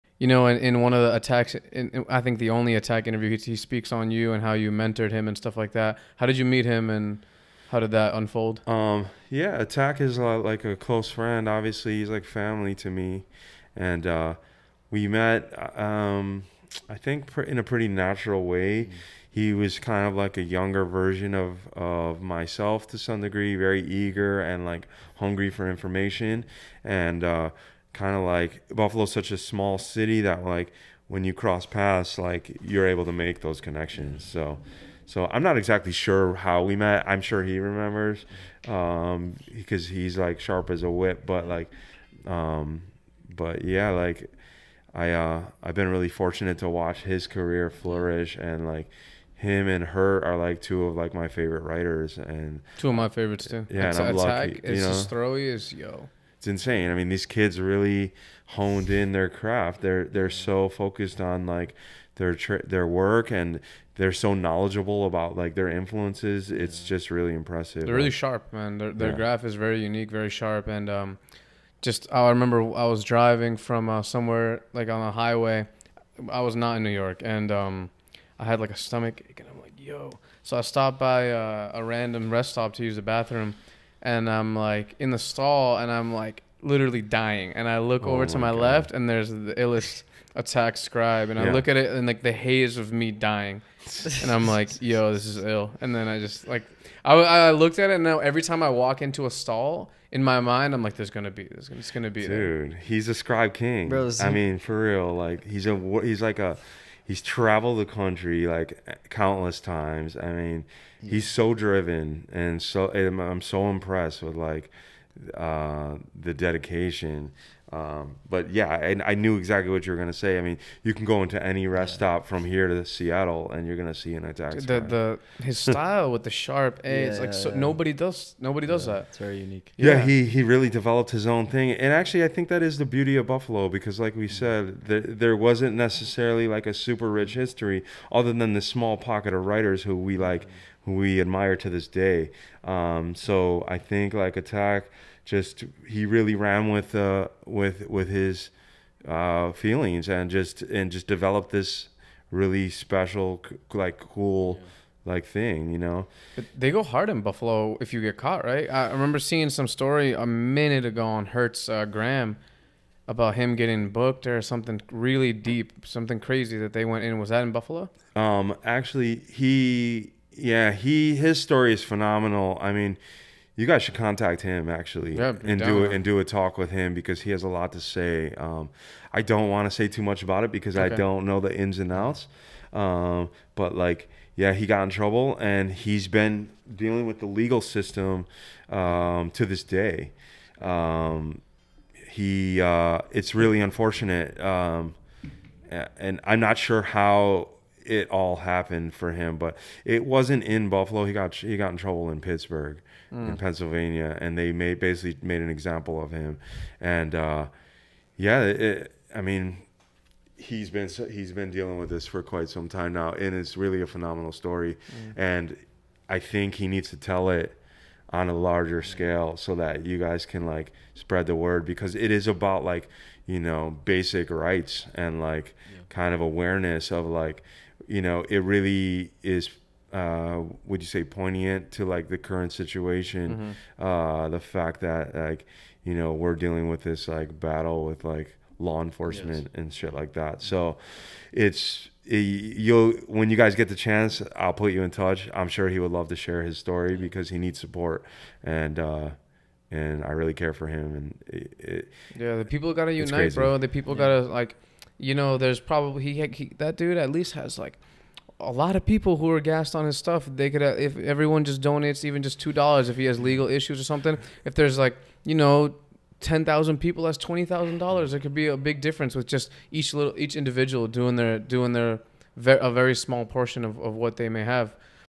Lions speaks on Atak BF
From Ep.32 of the Angel and Z podcast. Fernando Lions is a revered graffiti writer and tattoo artist from Buffalo New York who has been living in Brooklyn for the past 19 years. We speak on how the passing of Zexor and Acne deeply affected him, the finite-nature of human life, mentoring Atak BF, how he opened up his own studio “Street Dreams” amidst the pandemic, designing a shirt for Supreme and releasing a jewelry project with Louison Fine. Available on apple podcasts, Spotify, google play, our website, Youtube and more.